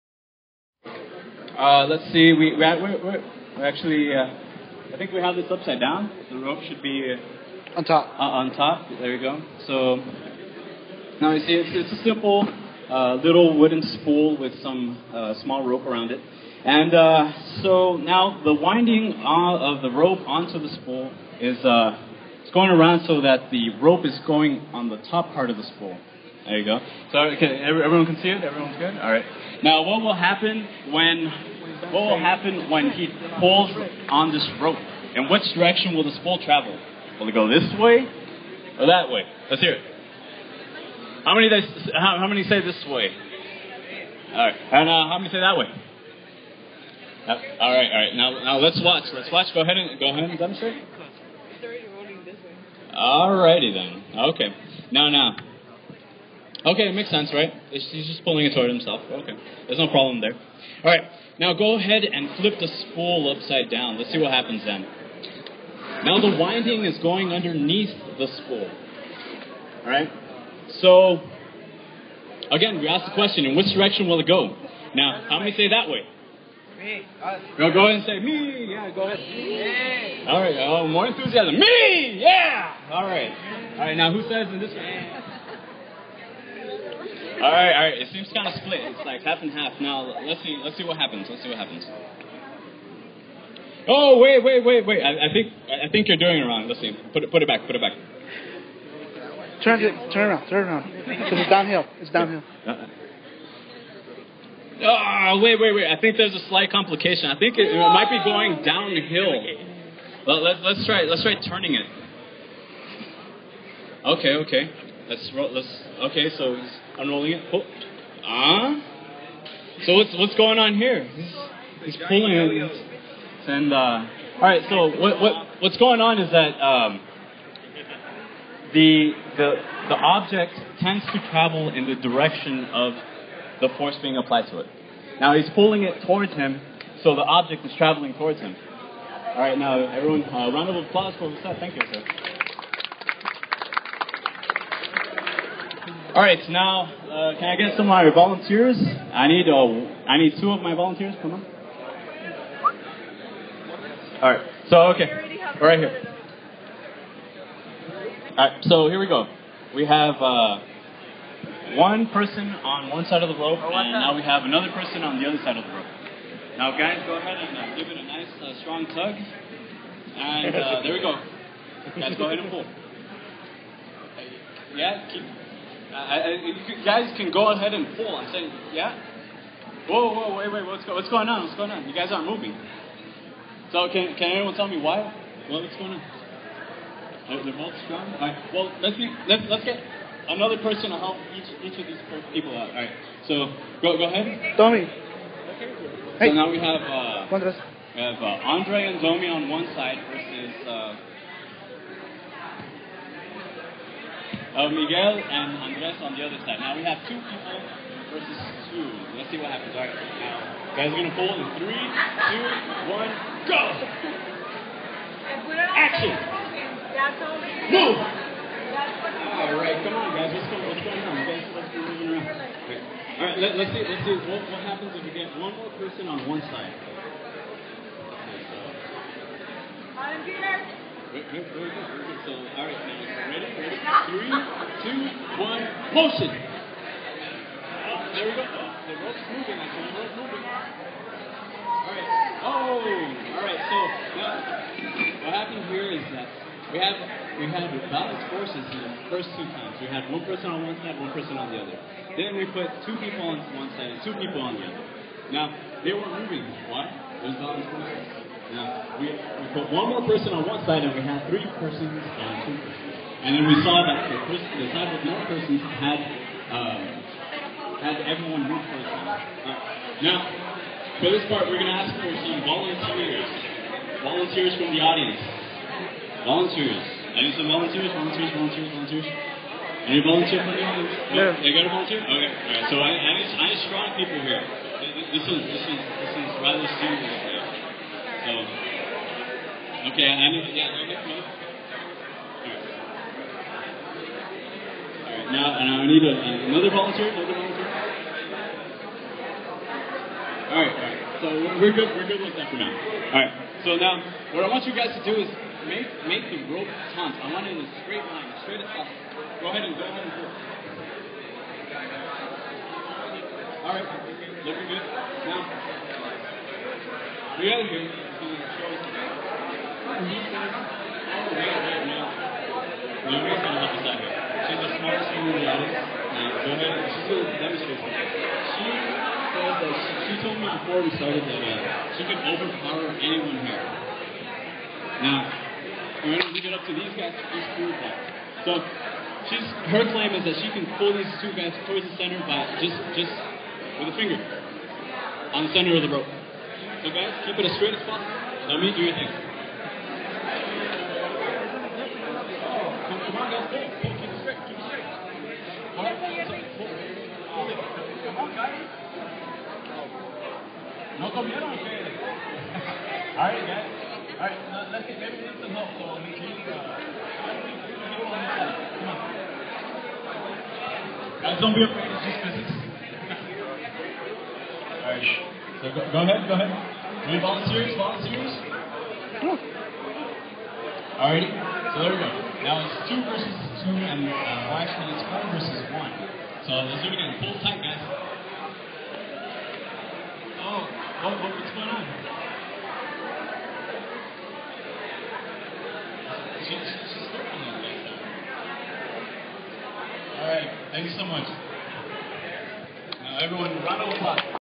SPS Physics Circus recorded live on 10/31/12 at 1:30 PM CDT
Recorded live from The University of Texas at Brownsvillw on 10/31/12 at 1:30 PM CDT - Captured Live on Ustream with the Ustream Mobile App